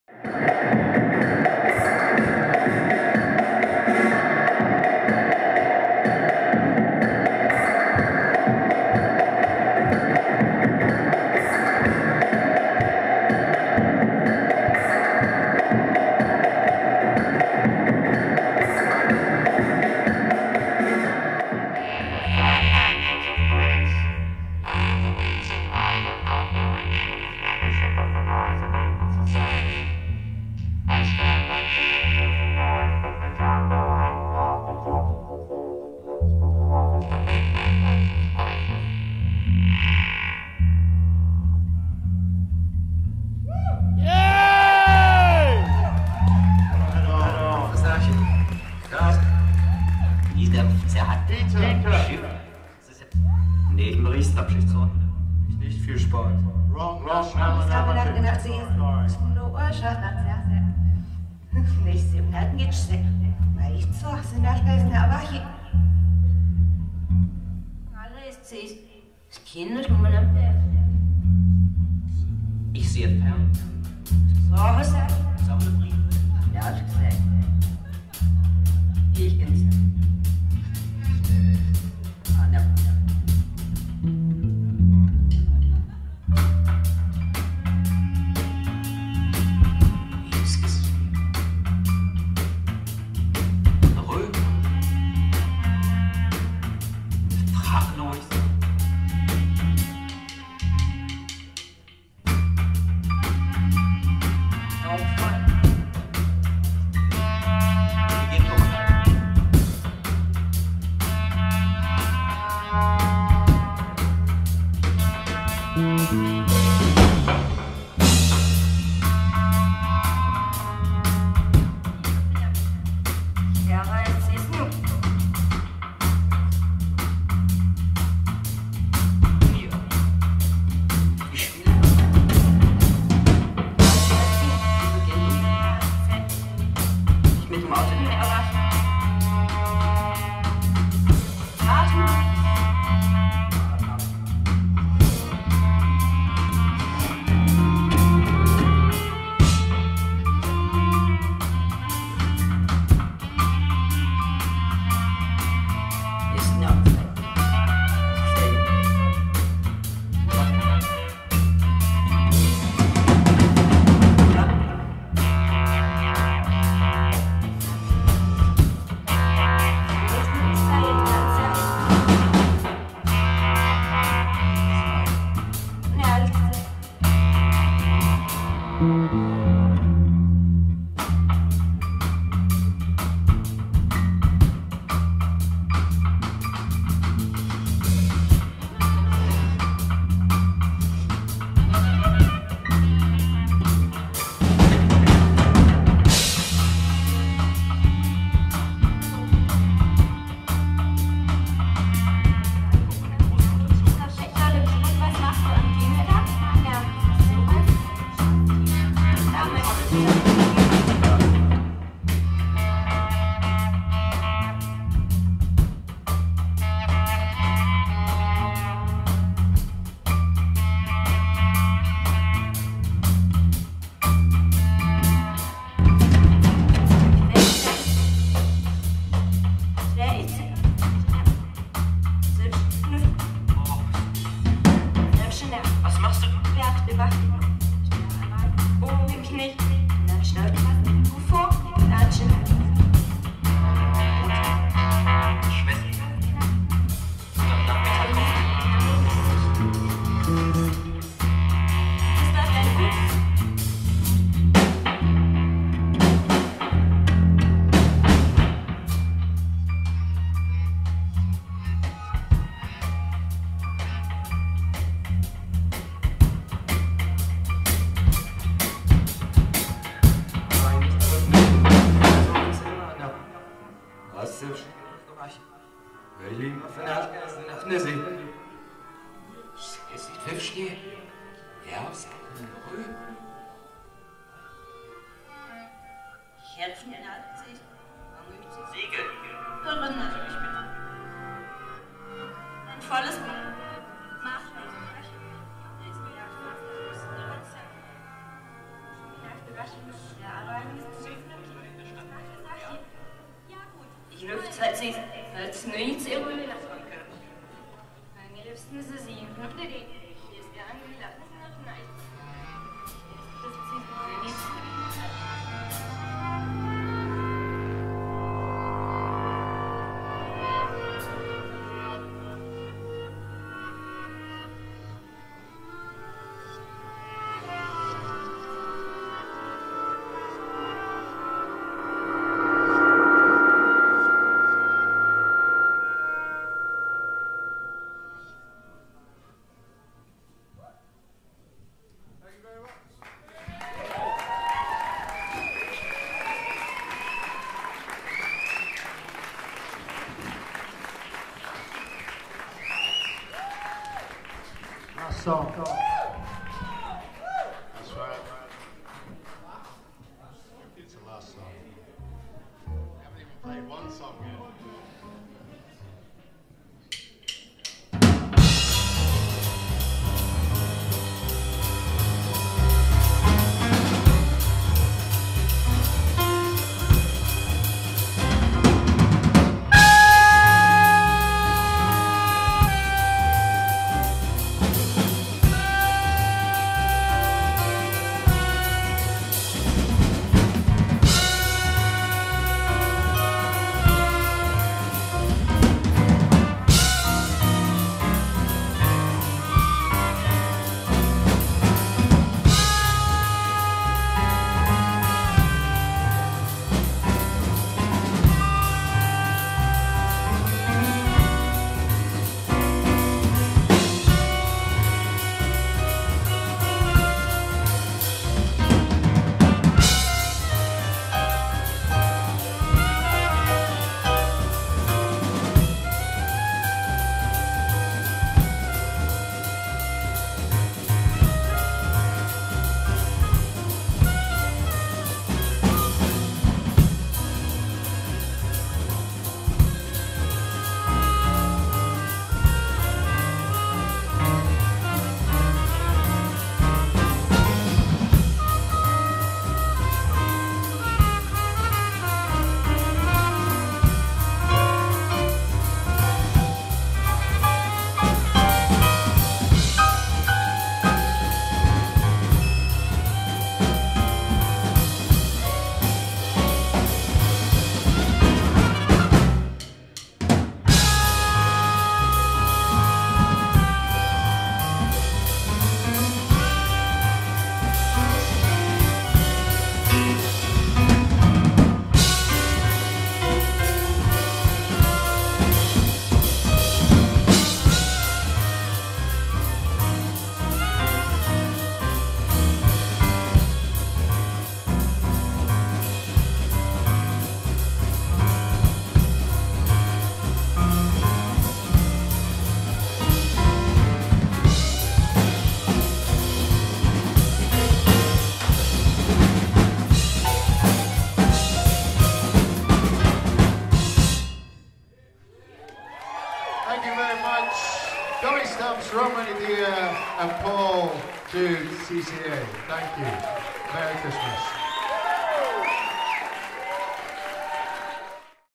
Tut Vu Vu - CCA, Glasgow 20.12.12
The video starts with Romany Dear, then Tut Vu Vu